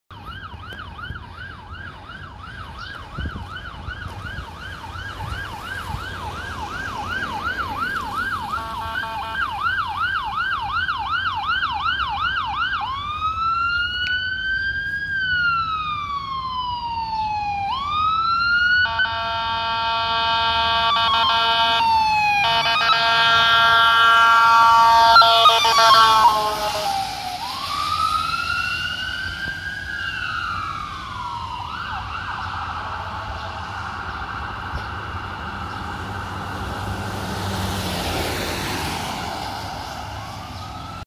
**HEAVY HORN** AMR Hartford Unit 932 Responding Priority 1 With Wail, Yelp, Priority and Heavy Horn!
Here you can see a Hartford CT amr unit responding priority 1 in West Hartford ct. Listen to how the emt driving lays on the horn and uses yelp and priority at the intersection! Enjoy this show!!